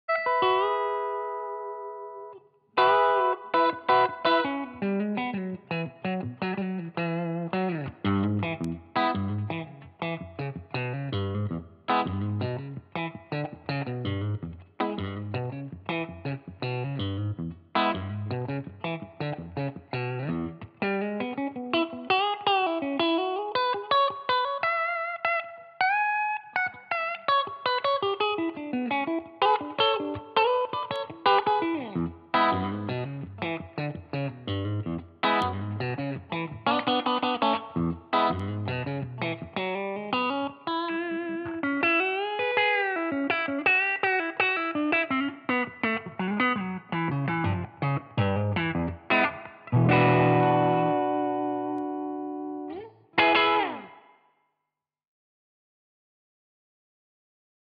Swiss Army Knife with great personality | PRS 509 Faded Whale Blue by Tom Groenendaal
One of the most versatile Paul Reed Smith guitars you'll find, or pretty much one of the most versatile guitars ever made in general. Sound great in every and any pickup position, and the coil split options make for a 9 (!) way pallet of wonderful tones. Fantastic guitar.